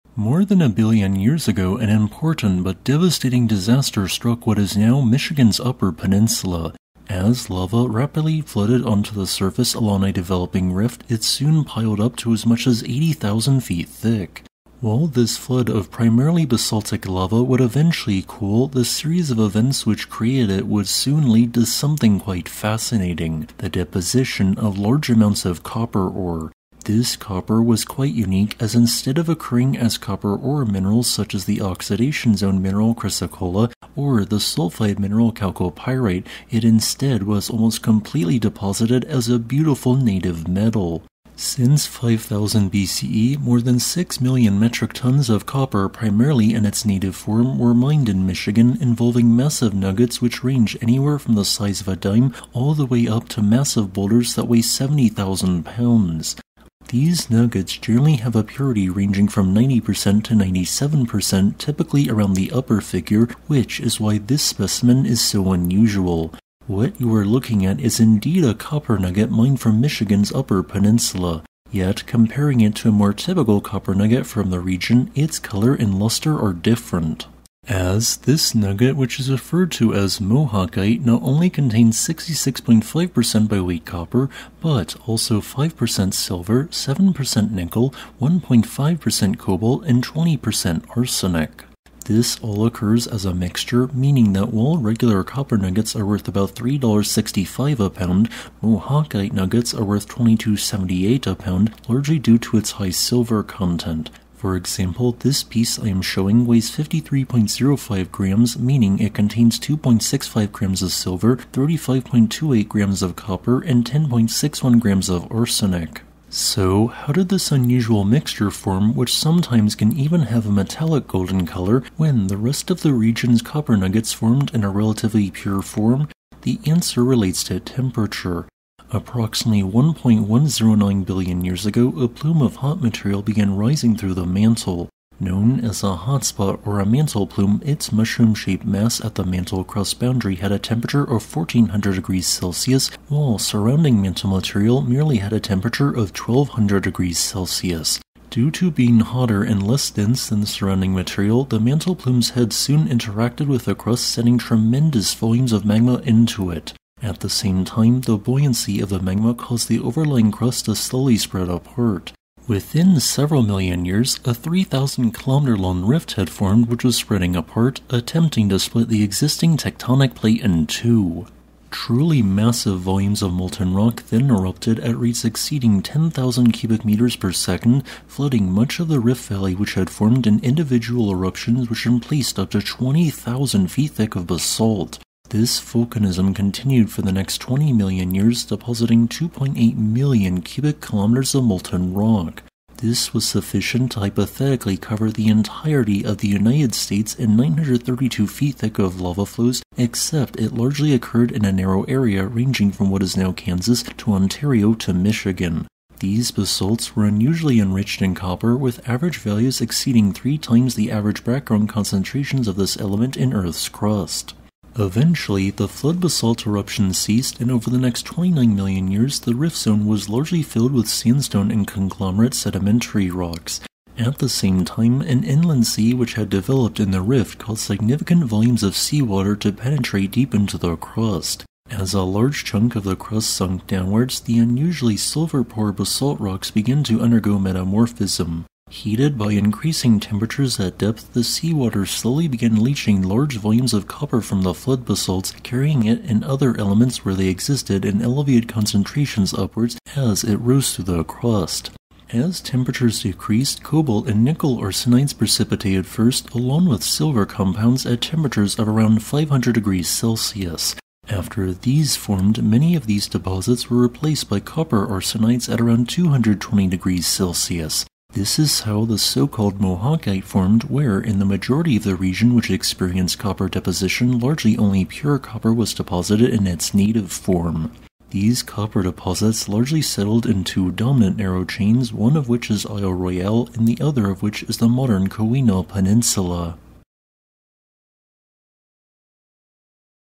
How Michigan's Ancient Volcanoes Formed Natural Bronze; A Geologic Oddity
In Michigan, ancient and massive outpourings of lava led to the creation of an unusual phenomena; natural bronze. This natural bronze can today be found at the Mohawk Mine, where unusually impure copper nuggets enriched in arsenic quite closely match the composition of ancient arsenical bronze. Yet, this was not created by humans but instead by natural geologic processes more than 1 billion years ago.

Note: Please do not trespass at or near the Mohawk Mine, as it is protected private property. This means no collecting!

Sources/Citations:
[2] Bodden, Thomas & Bornhorst, Theodore & Bégué, Florence & Deering, Chad. (2022). Sources of Hydrothermal Fluids Inferred from Oxygen and Carbon Isotope Composition of Calcite, Keweenaw Peninsula Native Copper District, Michigan, USA. Minerals. 12. 474.
[3] Prasanna Mahesh Gunawardana, Robert Moucha, Tyrone O. Rooney, Seth Stein, Carol A. Stein; North America's Midcontinent Rift magma volume: A coincidental rendezvous of a plume with a rift. Geology 2022; 50 (10): 1125–1129. doi
[4] The Google Earth KMZ file used in this video used to show the Geologic Map of Michigan was downloaded from: U.S. Geological Survey, Michigan geologic map data,

0:00 A Flood of Lava
1:18 Mohawkite Composition
2:09 Mantle Plume
2:52 Flood Basalts
3:38 Copper Formation